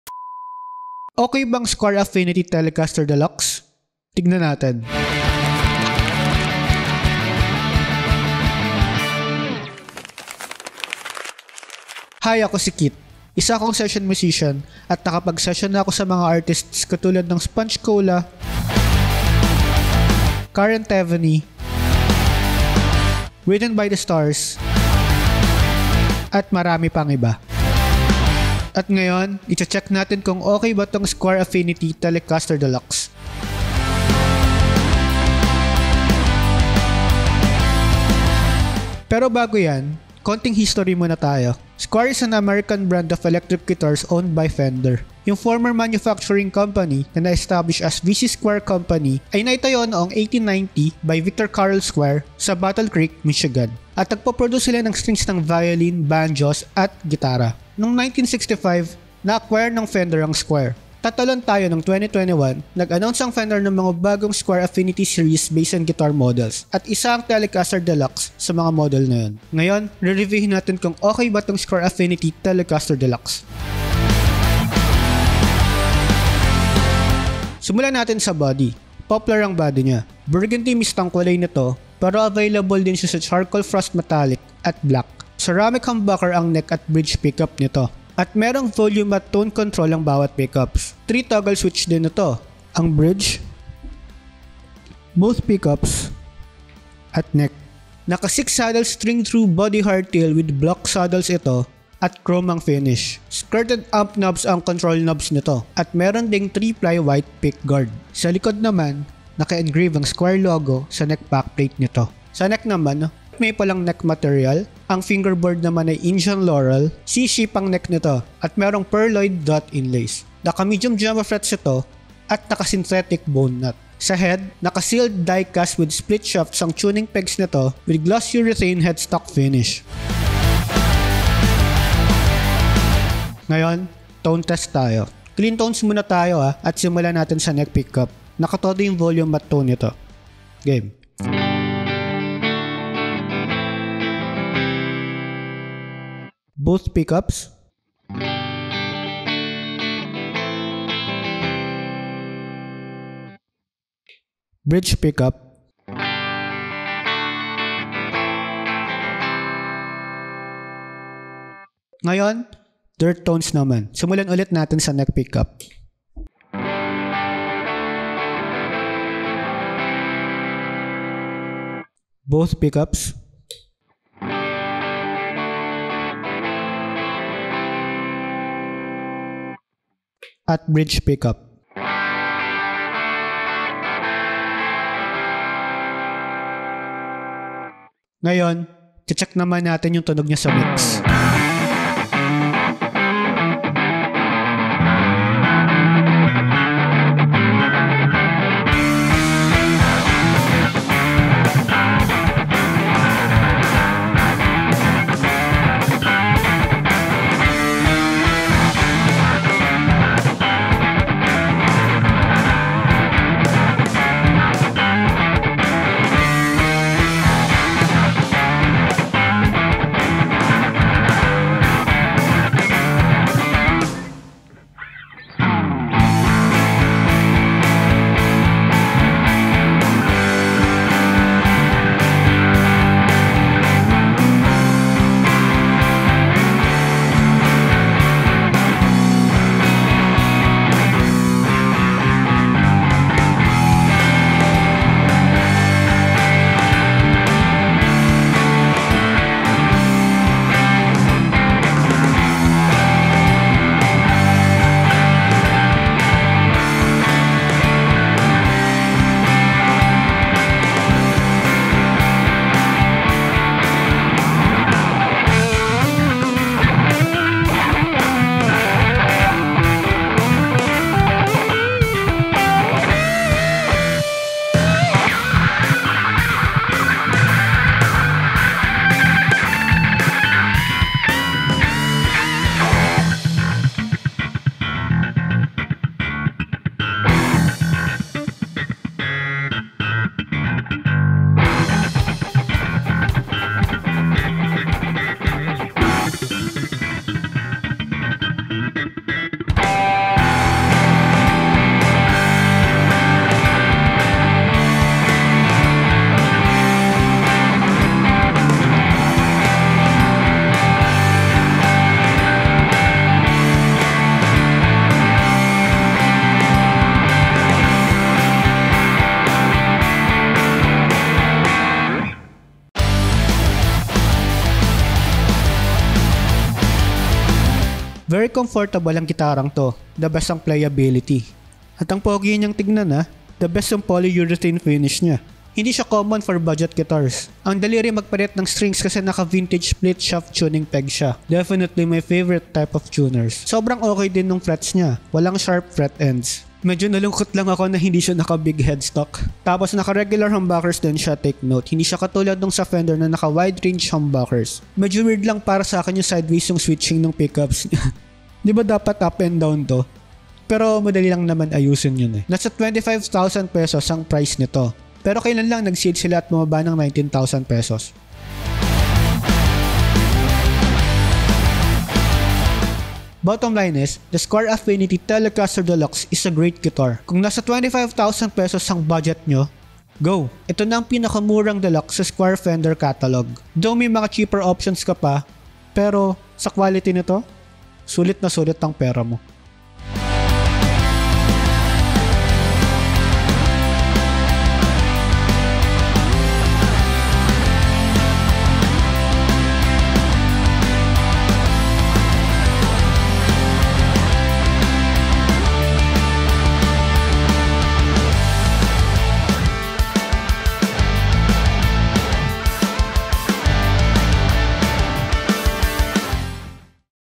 SULIT BA ANG SQUIER AFFINITY TELECASTER DELUXE?
Sulit ba ang Squier Affinity Telecaster Deluxe? Tara, icheck natin kung sulit ba o hindi